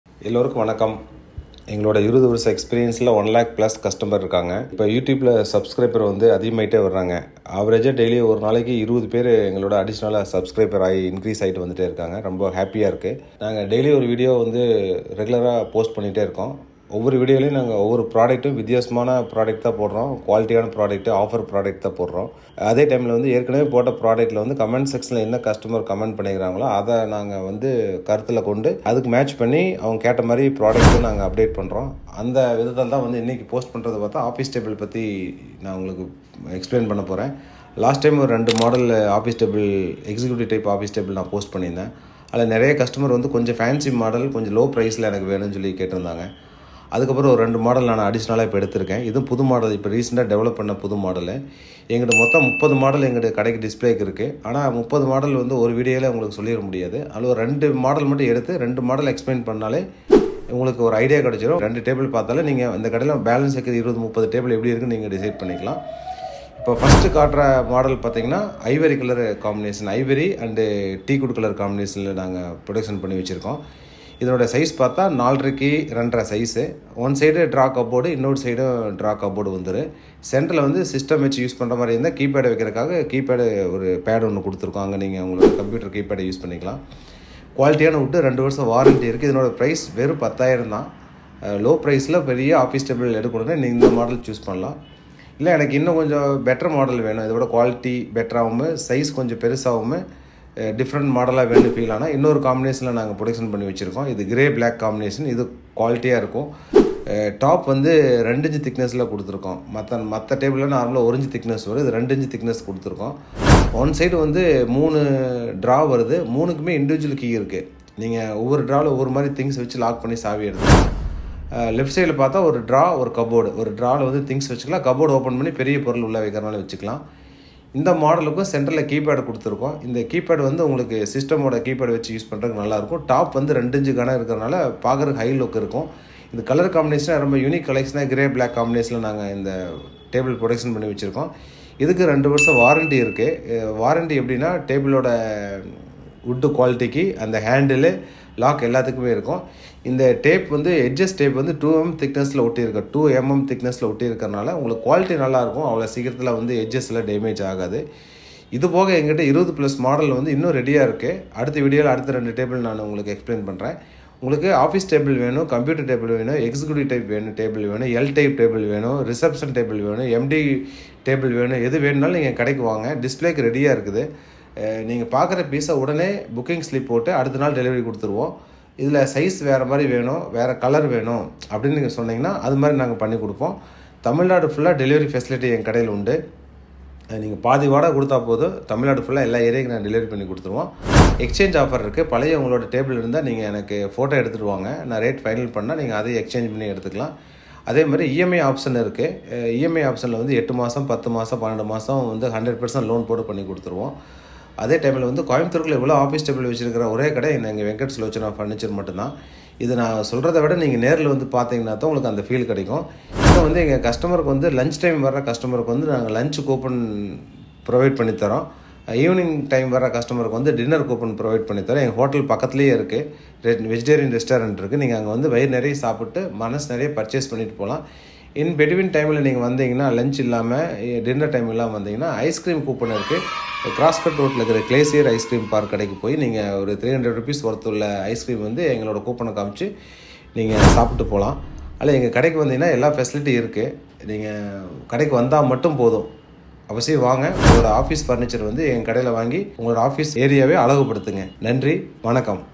Low cost Furniture in Coimbatore | Office table,pooja stand,wardrobe,computer table
Low cost Furniture in Coimbatore Office table Now @9,999/- only VENKAT SULOCHANA FURNITURE” the most promising furniture shops in coimbatore, we have varity of furniture’s in Luxury Sofa’s, wardrobes, Office Furniture’s, TV units, Rolling chairs, Pooja racks, King & Queen Size cot and Mattress, Dining table with chairs and more